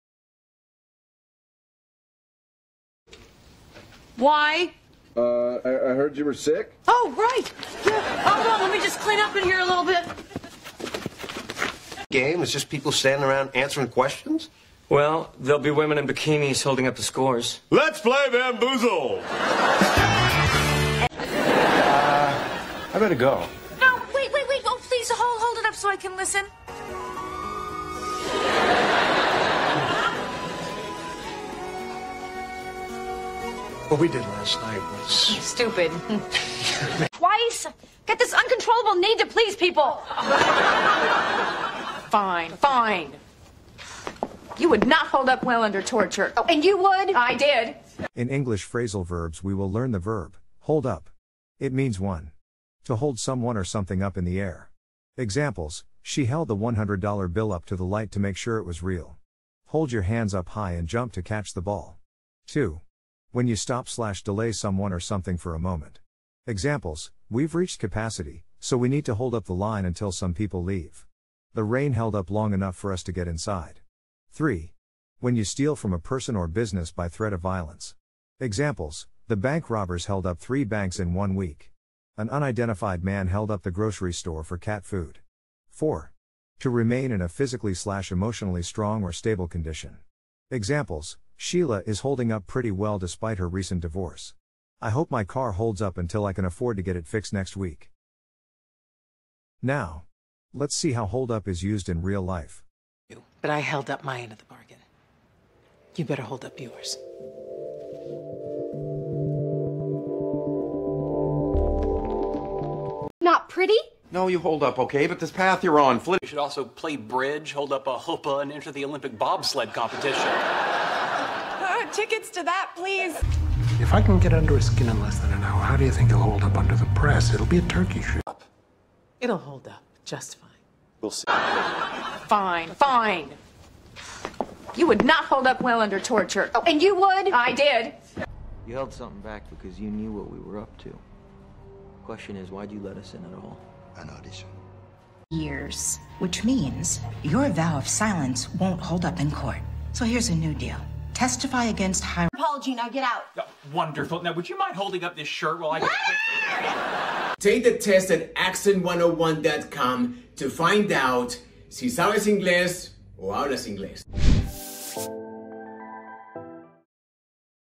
phrasal verbs: 'HOLD UP'
En esta Clase de inglés aprenderemos el phrasal verb HOLD UP con ejemplos de series de televisión

HOLD UP significa:

1. To hold someone or something up in the air.
Examples: She held the 100 dollar bill up to the light to make sure it was real. Hold your hands up high and jump to catch the ball.
2. When you stop/delay someone or something for a moment.
Examples: We’ve reached capacity, so we need to hold up the line until some people leave.  The rain held up long enough for us to get inside.
3. When you steal from a person or business by threat of violence.
Examples: The bank robbers held up three banks in one week. An unidentified man held up the grocery store for cat food.
4. To remain in a physically/emotionally strong or stable condition.
Examples: Sheila is holding up pretty well despite her recent divorce. I hope my car holds up until I can afford to get it fixed next week.

Let's see how 'HOLD UP' is used in Advanced English Phrasal Verbs:

Infinitive: Hold Up
Present Tense: Hold Up/ Holds Up
-ing Form: Holding Up
Past Tense: Held Up
Past Participle: Held Up